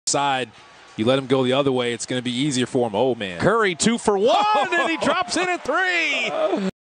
Steph Curry 38 footer!!! From deep!!
Curry showing off his unlimited range badge to the world.